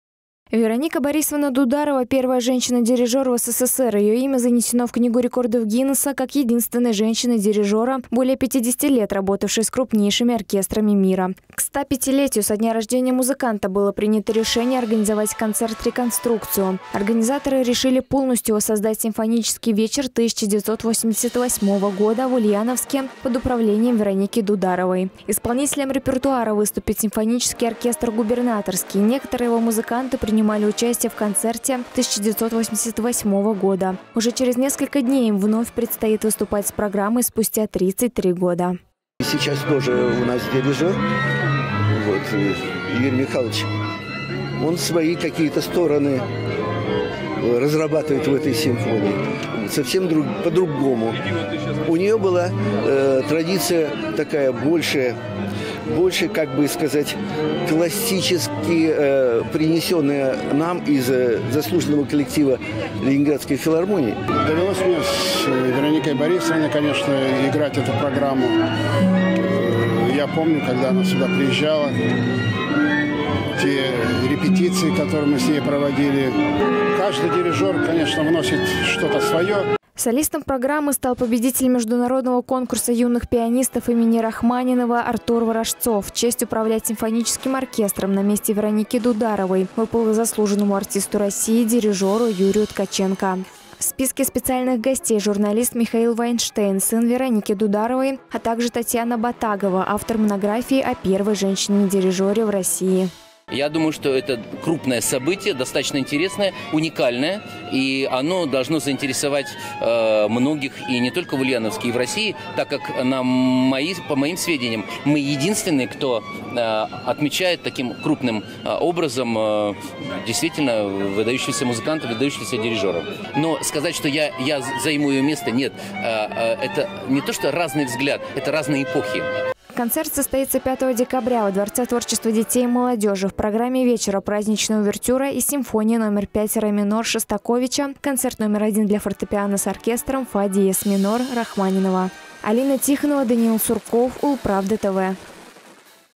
В Ульяновске воссоздадут концерт 1988 года
Заслуженный артист России Юрий Ткаченко выступит с Ульяновским государственным симфоническим оркестром «Губернаторский». По задумке организаторов, концерт-реконструкция состоится к 105-летию выдающегося дирижера современности, народной артистки СССР Вероники Борисовны Дударовой.